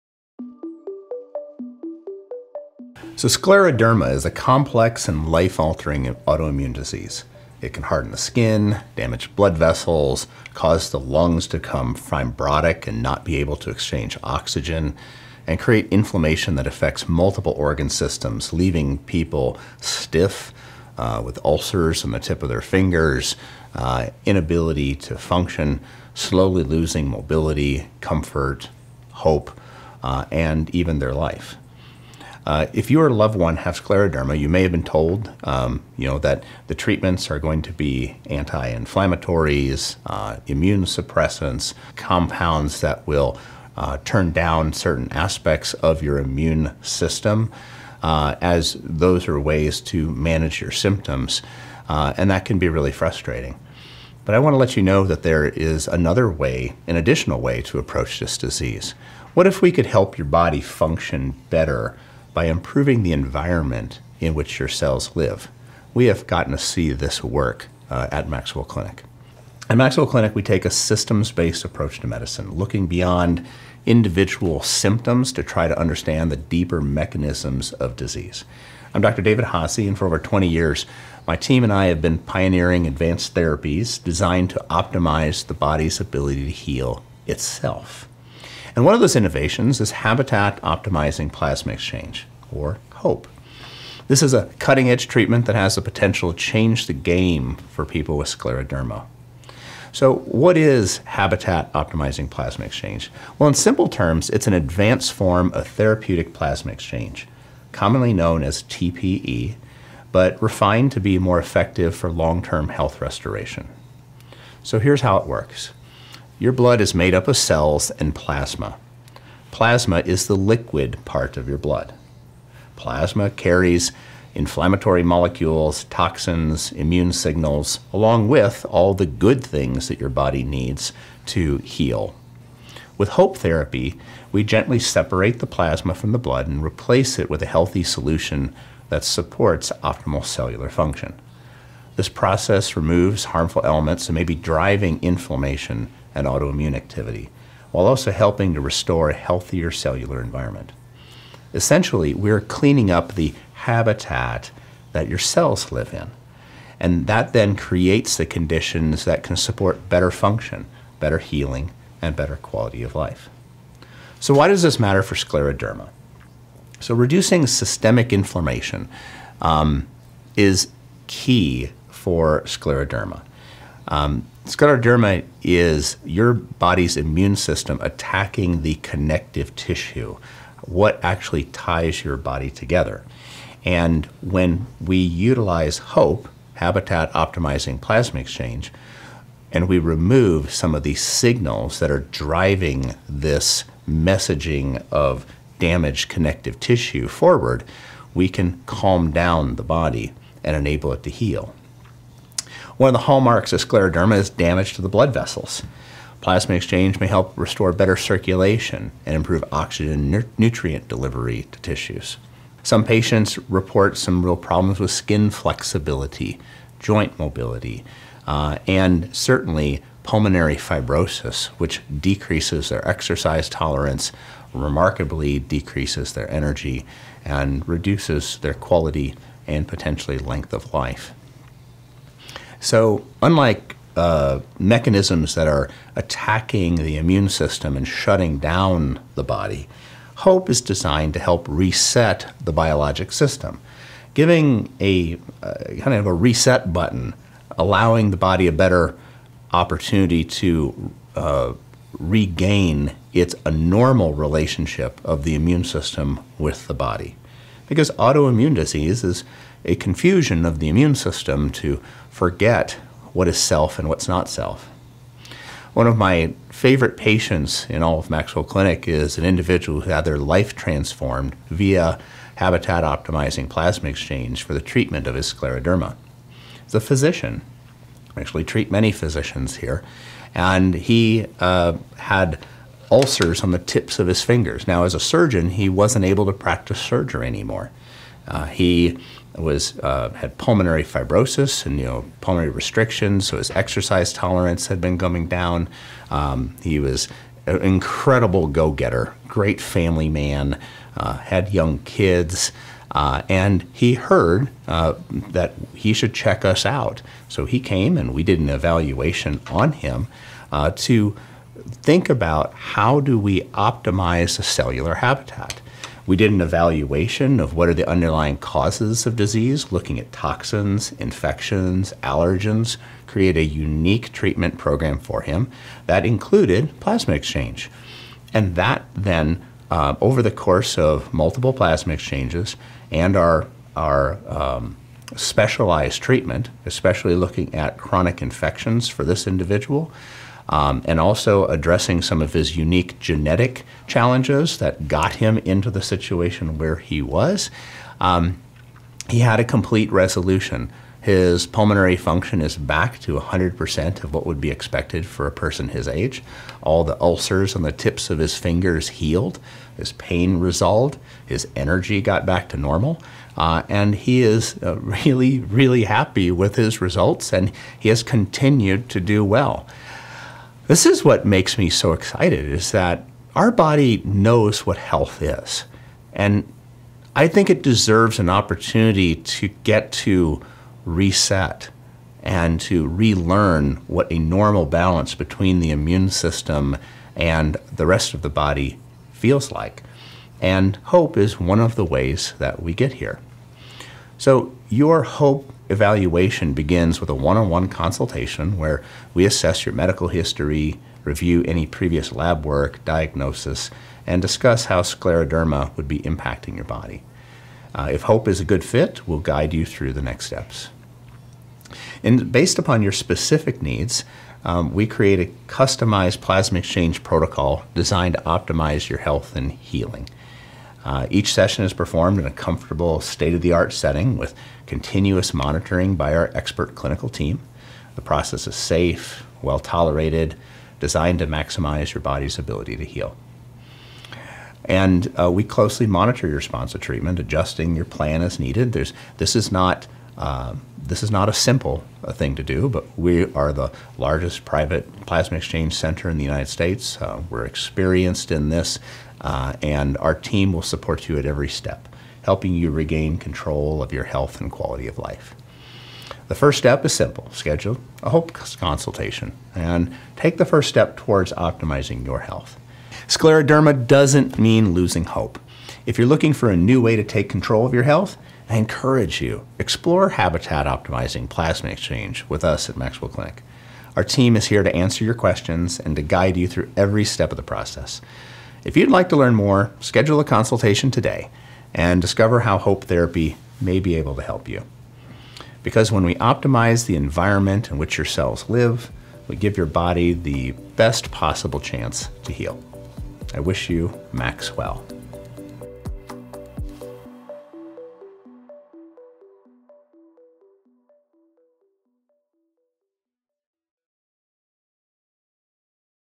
HOPE Plasma Exchange: A Breakthrough Treatment for Scleroderma | David Haase, MD
Scleroderma is life-altering—but there is HOPE.

At MaxWell Clinic, we take a systems-based approach to chronic disease, looking beyond just symptom management. Our Habitat Optimizing Plasma Exchange (HOPE) is an advanced plasma exchange therapy designed to help reset the immune system, reduce inflammation, and restore optimal cellular function—giving your body the best possible conditions for healing.

👩‍⚕️ In this video, David Haase, MD explains:
 ✅ Why standard treatments for scleroderma focus only on symptom management
 ✅ How HOPE goes beyond traditional plasma exchange to optimize healing
 ✅ The role of inflammation, blood vessel damage, and immune dysfunction in scleroderma
 ✅ How one patient—once unable to practice surgery—fully regained health & function
 ✅ What to expect from a HOPE consultation & customized treatment plan

💡 HOPE is more than just plasma exchange—it’s a personalized, science-driven approach designed to optimize the environment where your cells live, giving your body the best chance to heal.

Your health is the most valuable asset you have—don't wait and take control of your health today!

00:00 What is Scleroderma?
00:41 Current Treatments & Their Limitations
01:16 A New Approach: HOPE Therapy
02:29 Why Plasma Exchange Matters for Scleroderma
04:38 Real Patient Success Story
06:45 What to Expect from HOPE Therapy